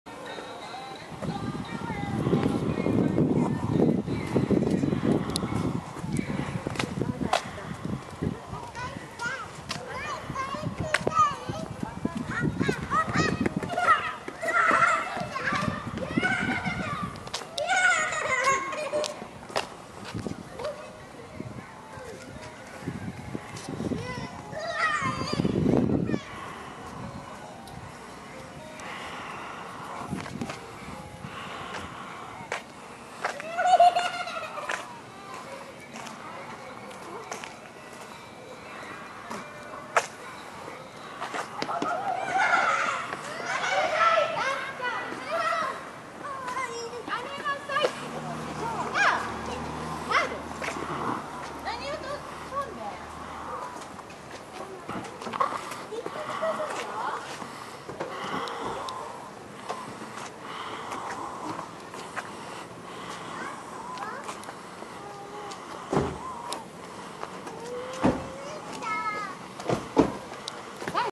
岡山市 人気 イルミネーション 2014/12/21
岡山市にある、家　イルミネーションです。毎年欠かさず見に行っています。